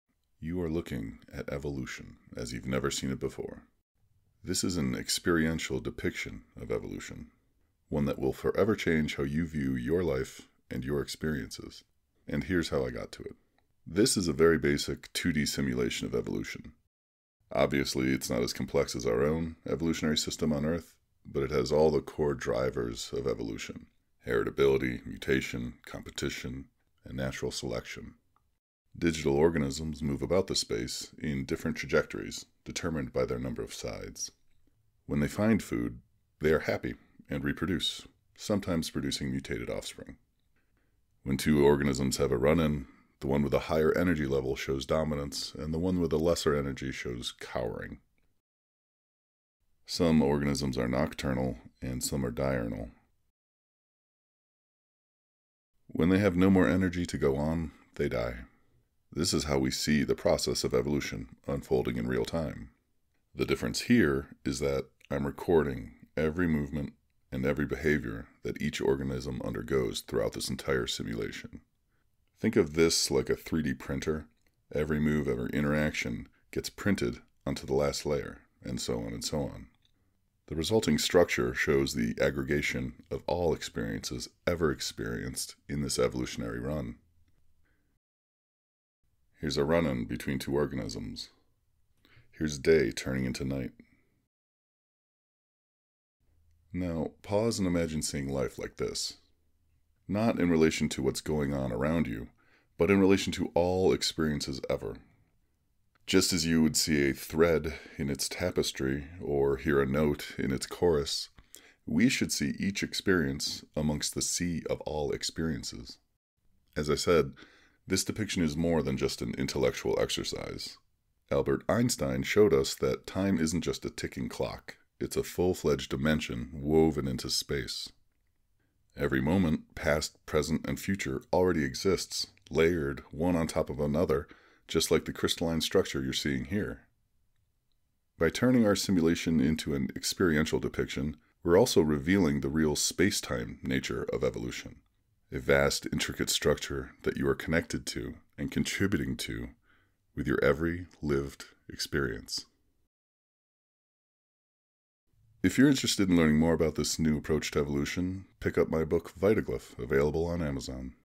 A New View of Evolution
This video shows what Evolution would look like in the context of all space-time and what it means for our own lives.